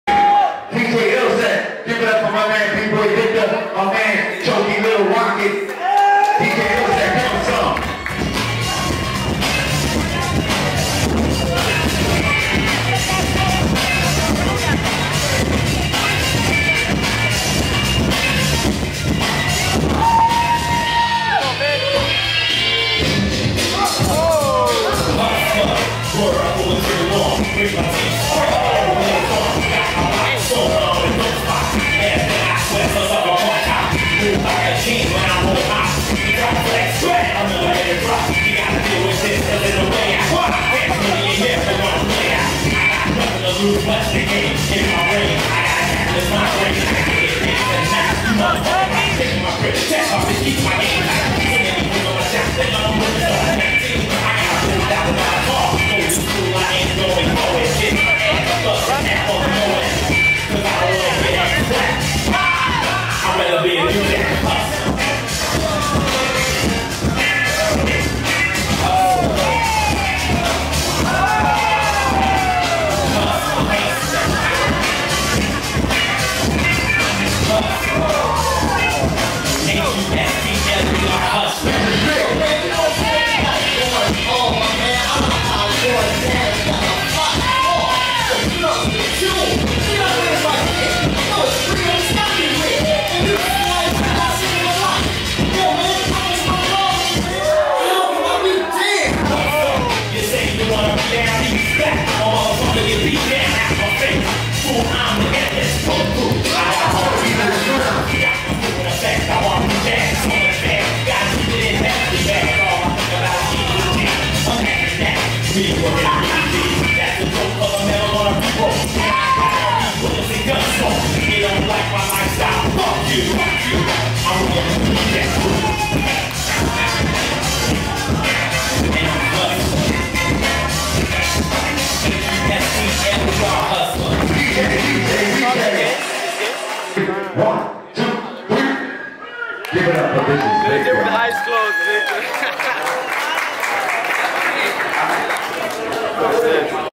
Choky vs Victor | Top 8 | Never Canceled 2020 - BreakinMIA Media
Never Cancelled hosted by - Street Masters Crew

FOLLOW - @BreakinMIA